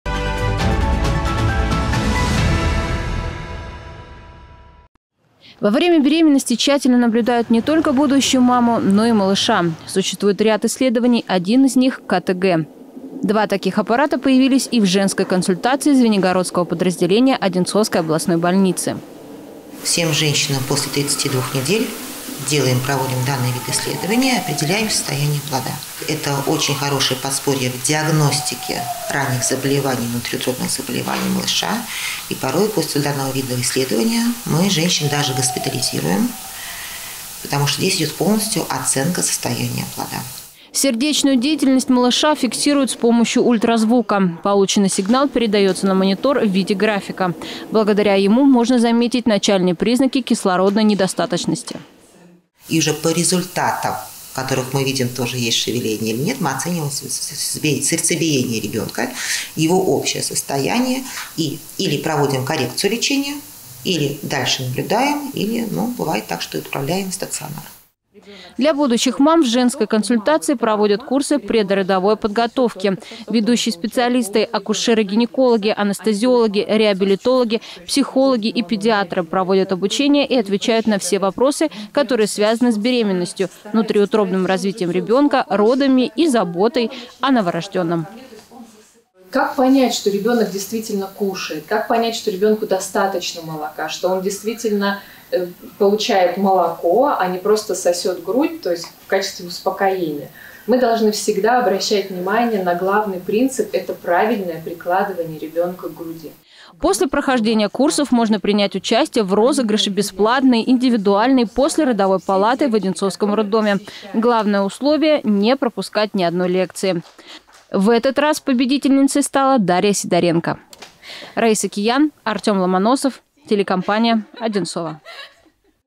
Оборудование в женской консультации в Звенигороде
В Звенигородской женской консультации теперь можно пройти обследование на аппарате КТГ. Этот метод позволит врачу зарегистрировать шевеление ребёнка, а будущим мамам - услышать сердцебиение малыша.